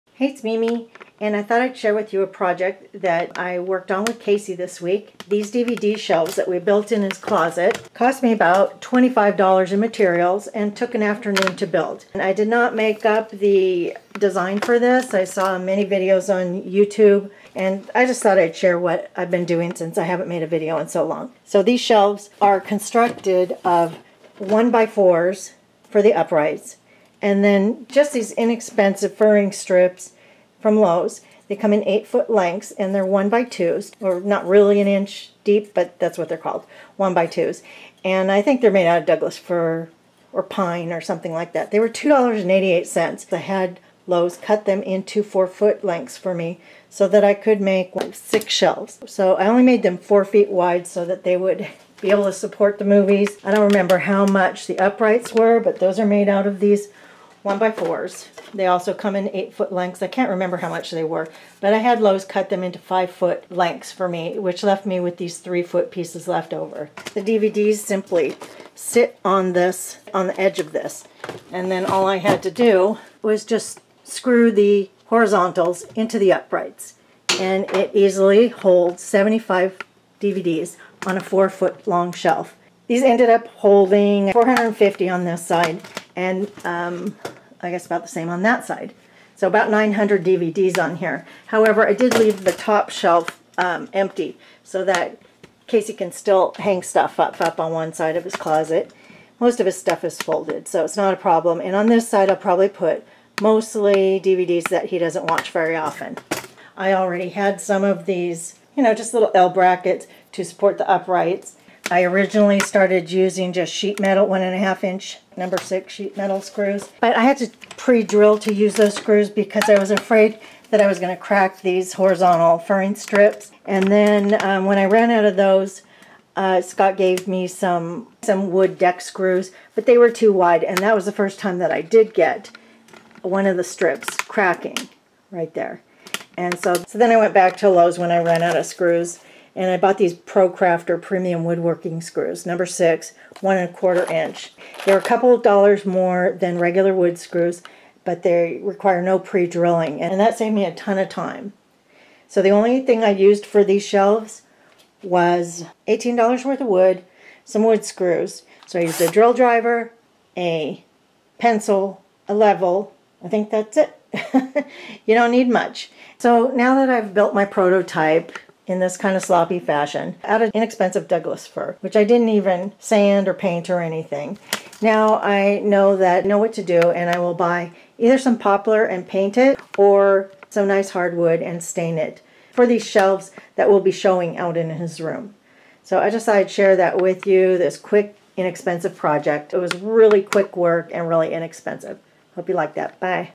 DIY DVD Shelves - Quick and Cheap DVD Storage Shelf
Inexpensive DVD Storage you can build for less than $25 in just one afternoon.  Have Lowes or Home Depot cut all the wood for you and just screw it together in a hour or two!